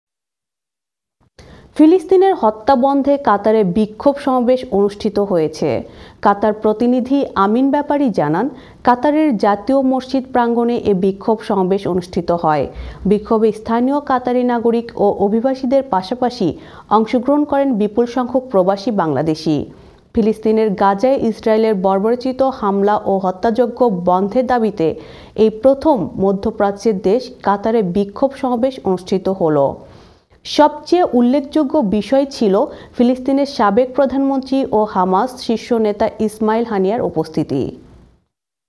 ফিলিস্তিনের হত্যা বন্ধে কাতারে বিক্ষোভ সমাবেশ অনুষ্ঠিত হয়েছে
ফিলিস্তিনের হত্যা বন্ধে কাতারে বিক্ষোভ সমাবেশ অনুষ্ঠিত হয়েছে। কাতার প্রতিনিধি আমিন ব্যাপারী জানান, কাতারের জাতীয় মসজিদ প্রাঙ্গণে এ বিক্ষোভে সমাবেশ অনুষ্ঠিত হয়। বিক্ষোভে স্থানীয় কাতারি নাগরিক ও অভিবাসীদের পাশাপাশি অংশগ্রহণ করেন বিপুলসংখ্যক প্রবাসী বাংলাদেশি। ফিলিস্তিনের গাজায় ইসরায়েলের বর্বরোচিত হামলা ও হত্যাযজ্ঞের বন্ধের দাবিতে, এই প্রথম মধ্যপ্রাচ্যের দেশ কাতারে বিক্ষোভ সমাবেশ অনুষ্ঠিত হল। সবচেয়ে উল্লেখযোগ্য বিষয় ছিল, ফিলিস্তিনের সাবেক প্রধানমন্ত্রী ও হামাস শীর্ষ নেতা ইসমাইল হানিয়ার উপস্থিতি।
iON TV,
Sky 782